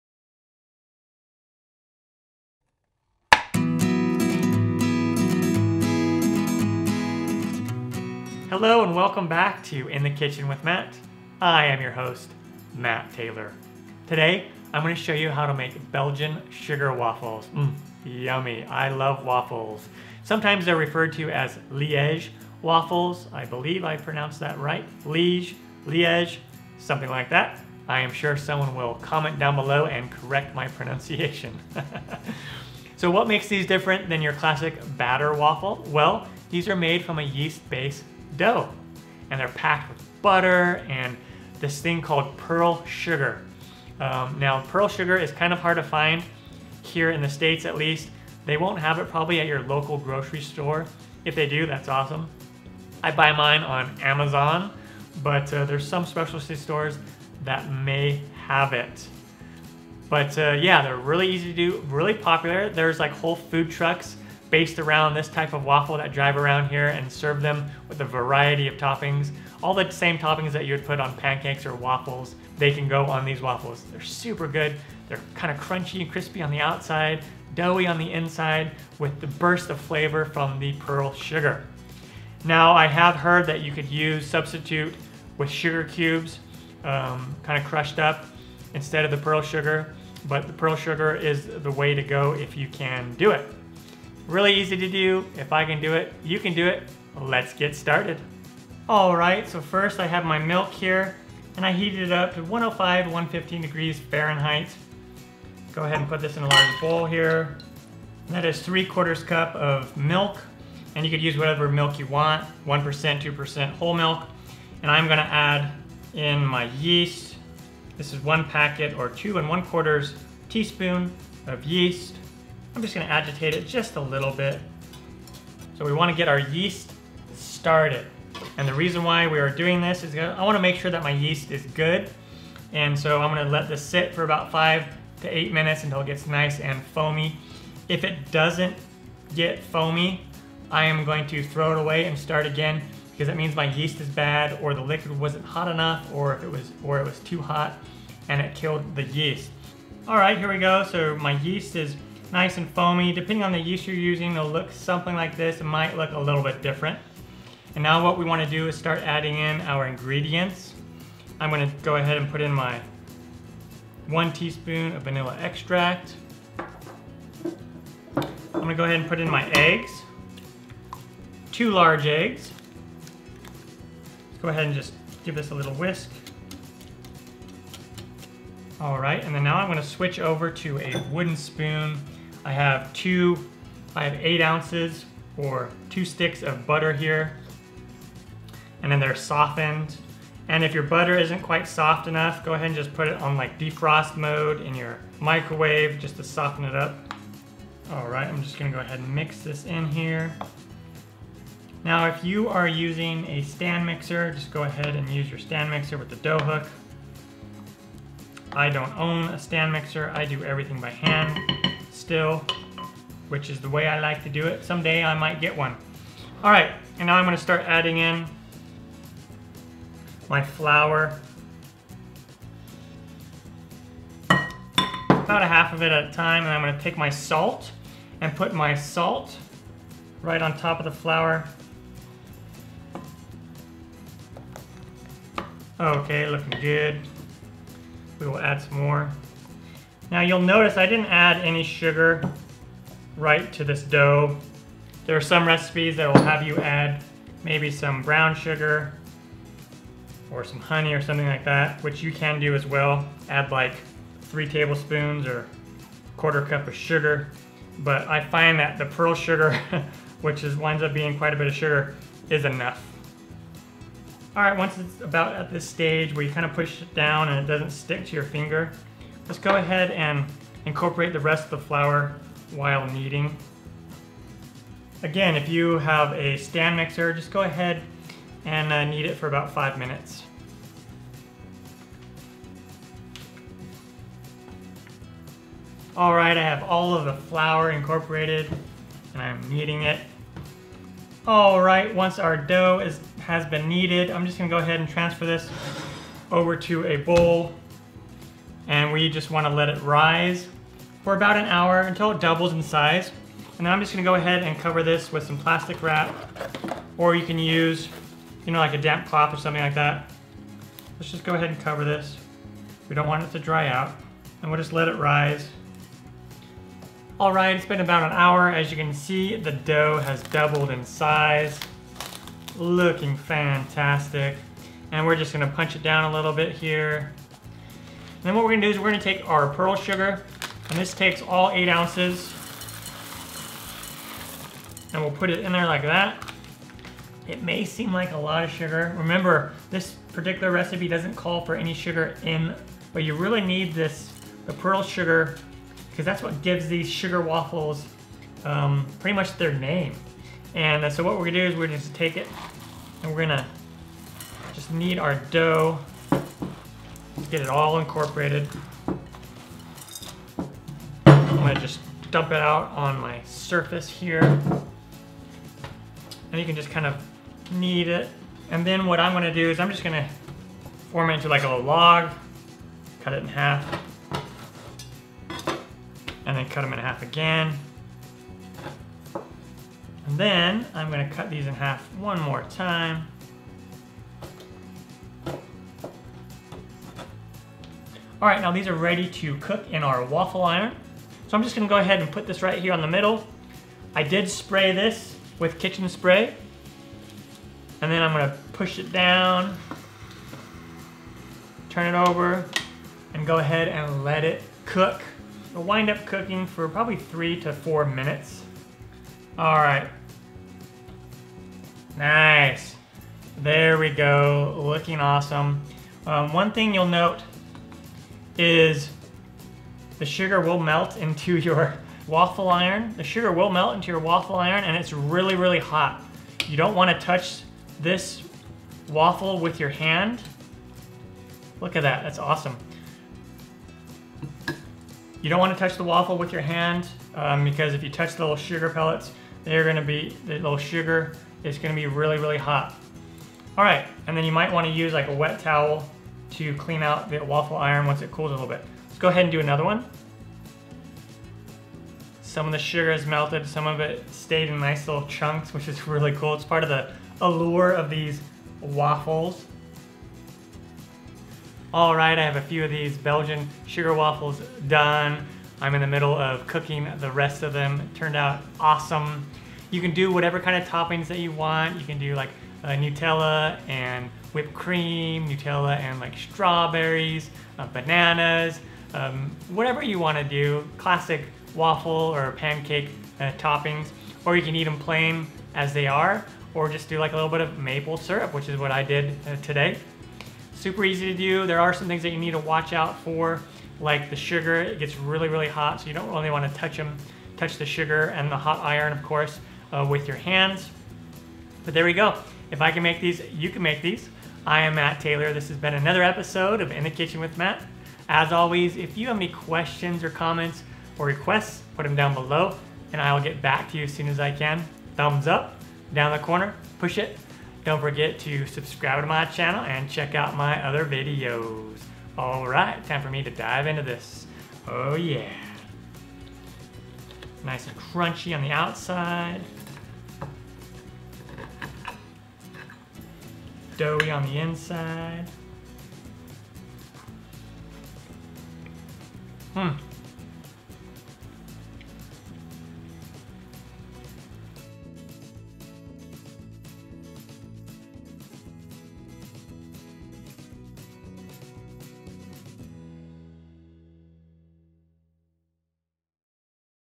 Belgian Sugar Waffles | Easy Liege Waffle Recipe
In this episode of In The Kitchen with Matt, I will show you how to make Belgian Sugar Waffles, also sometimes called Liege Waffles. They are very popular here in Phoenix where I live, we have Waffle food trucks that drive around selling these amazing waffles, with all kinds of different toppings. Belgian Sugar Waffles recipes are different then a classic batter waffle recipes. They are made from a yeast based dough instead and have an ingredient called pearl sugar in them. The name "Sugar" waffle comes from the pearl sugar. If you have never tasted one of these before, you are missing out. :) So now you can make your own, if I can make them, you can make them, let's get started.

Ingredients:
3 1/2 cups of all-purpose flour (420g)
2 1/4 tsp. of active dry yeast (1 packet of instant yeast will work too) (7g)
3/4 cup of warm milk (1%, 2%, Whole, etc. 100 to 115 F.) (177mL)
8 ounces or 1 cup, softened butter (226g)
2 large eggs
1/2 tsp. salt (2g)
1 tsp. vanilla extract (5ml)
1 8 ounce bag of Pearl Sugar (225g)

Tools:
waffle iron
wooden spoon
bowls
whisk
tongs or fork
dough cutter or knife
plastic wrap

Be very careful when cooking these waffles, the sugar gets really really hot! Don't touch the sugar in the waffles. :) And let them cool a bit before biting into them.